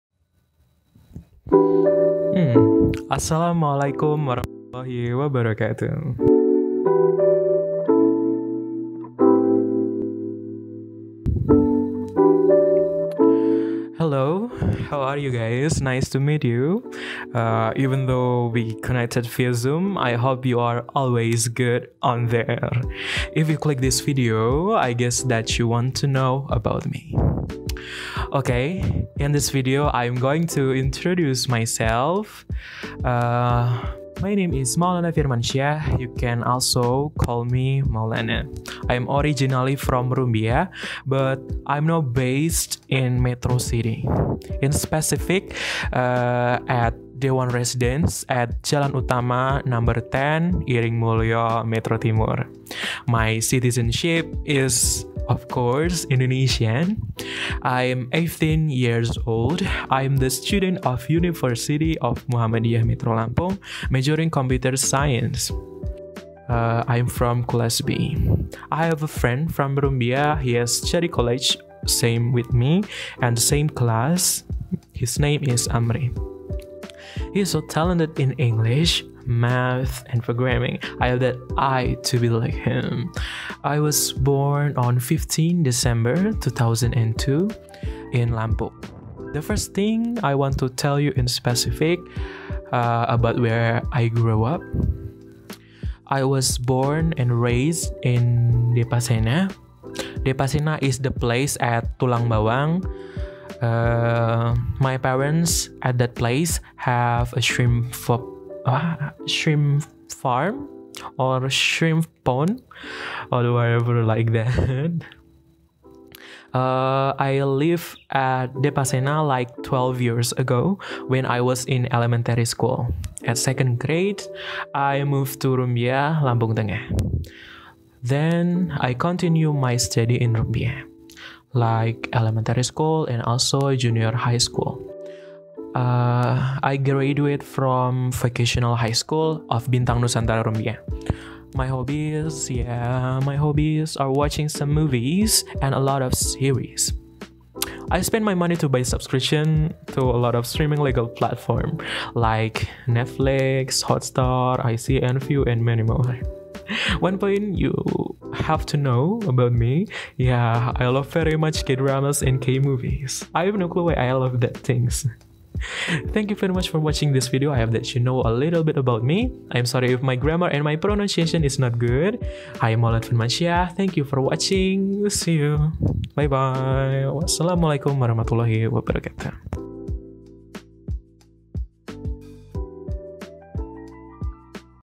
SELF INTRODUCTION | MAULANA FIRMANSYAH | 21430057
Tugas Perkenalan Bahasa Inggris
Nama: Maulana Firmansyah
NPM: 21430057
Prodi: S1 Ilmu Komputer
Dosen Pengampu: Yasmika Baihaqi, M.Pd.BI